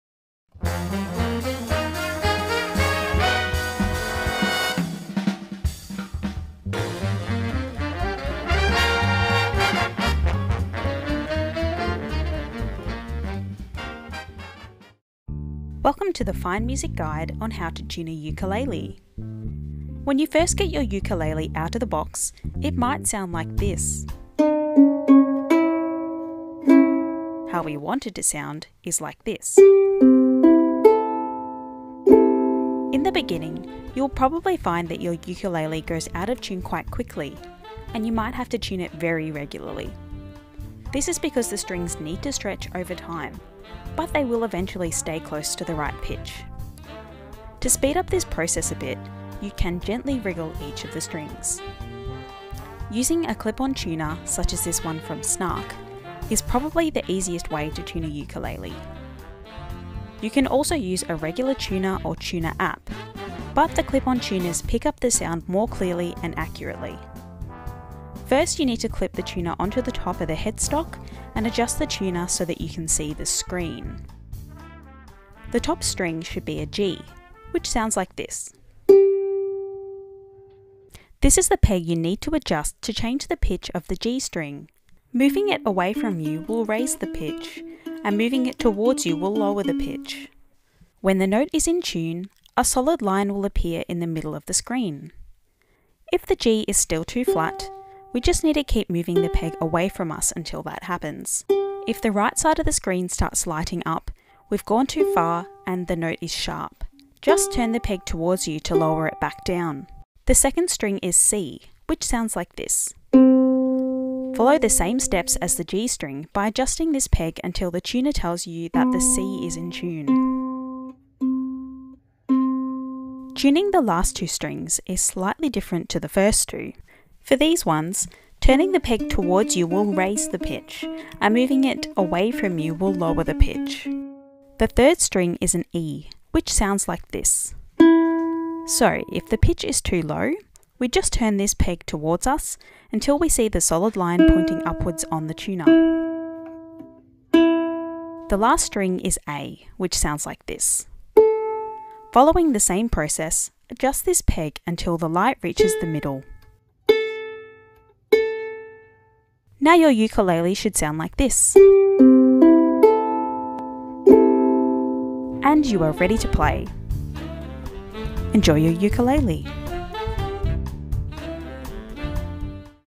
How to Tune Your Ukulele
Everything you need to know about tuning a ukulele!

Ukulele pictured:
Flight Travel Soprano Ukulele (black)

Photo at 0:27 by Alexander Krivitskiy on Unsplash